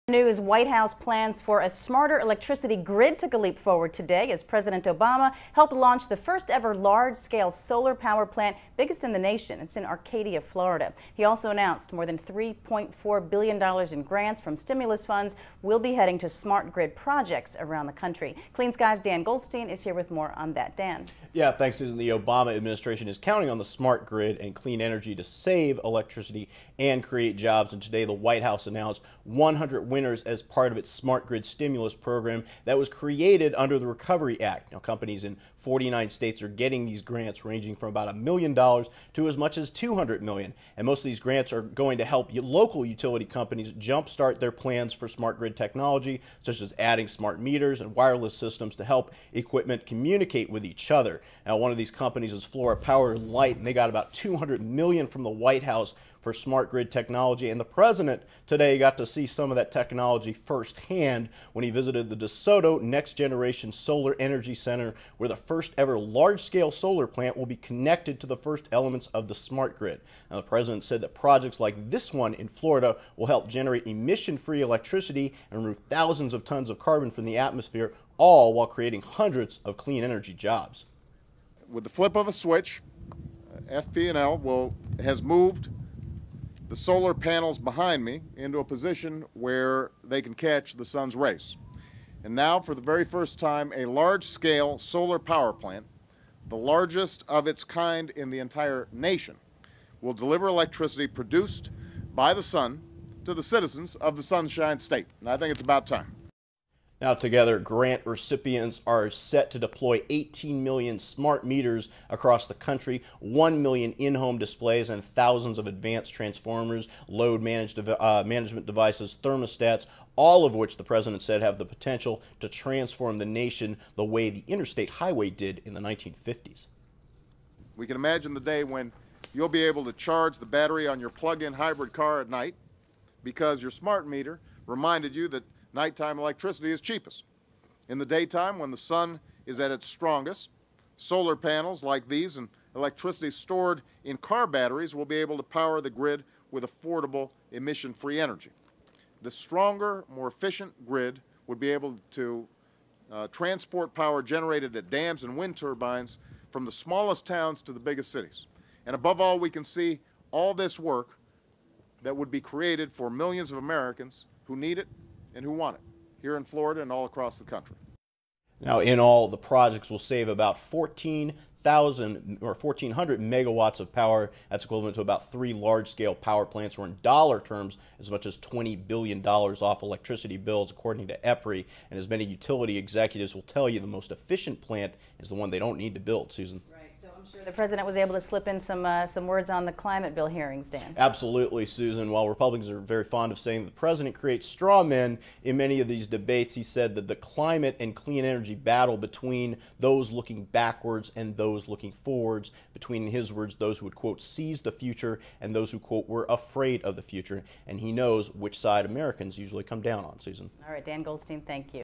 Obama Launches Solar Plant, Grants $3.4 Billion
President Barack Obama is making leaps and bounds in smart grid advancements, helping launch the nation's largest solar plant and allocating more than $3.4 billion for smart grid projects. Clean Skies News looks at where this money is going, and how these projects can affect both our grid and our economy.